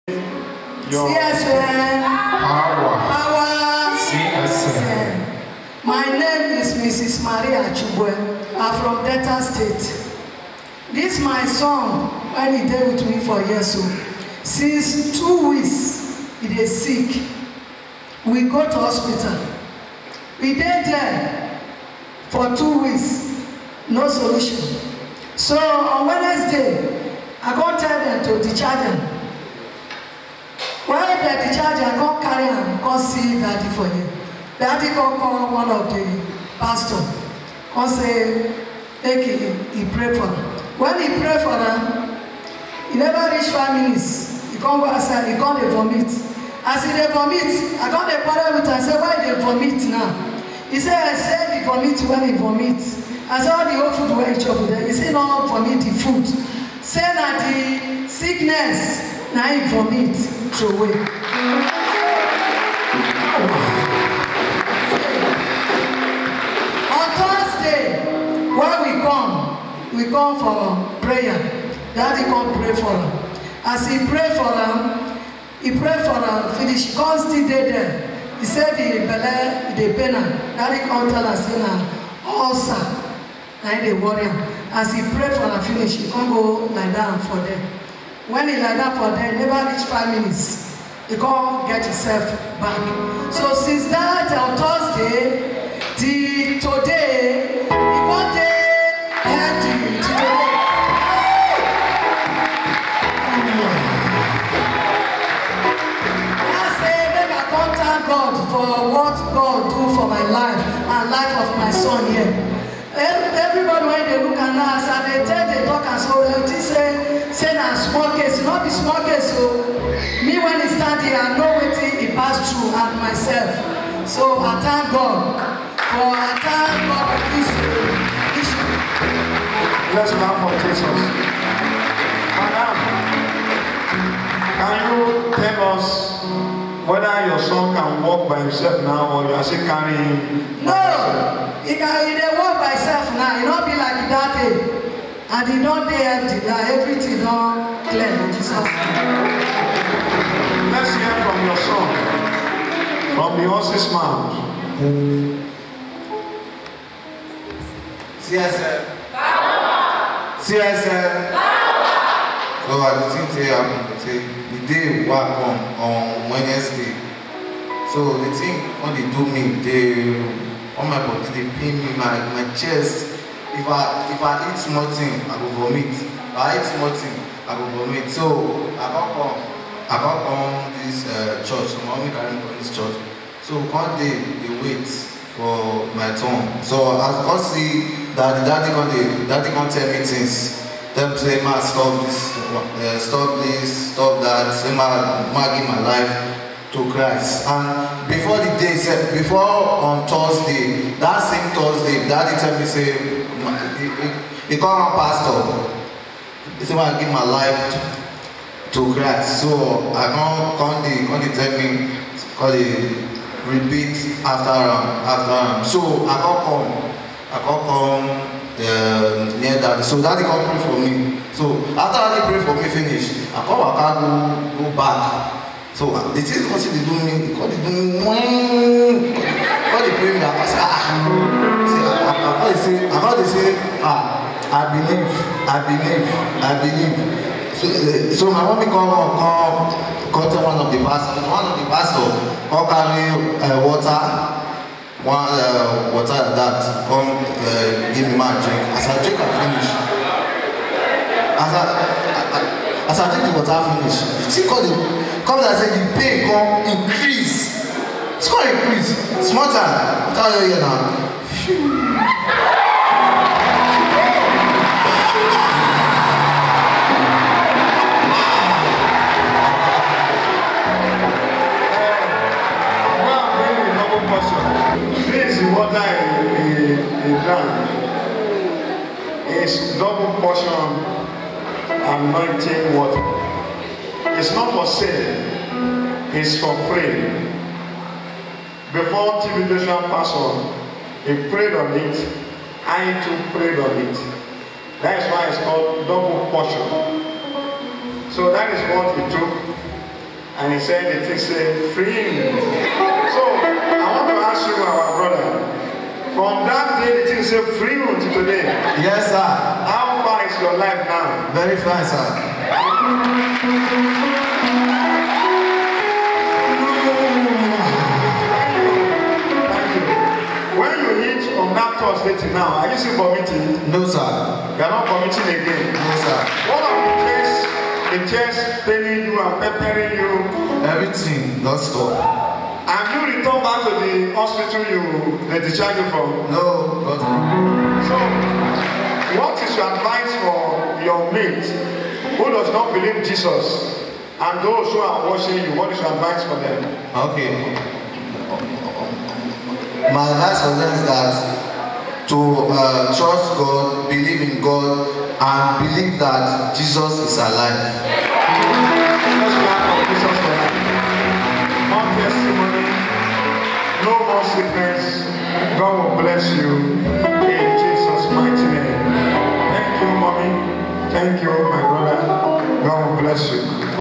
Testimony: I Vomited Out My Sickness
Watch and hear the great testimony of this woman on how God used Prophet A. O. Omokoje to delivered her son from terrible sickness he was afflicted with.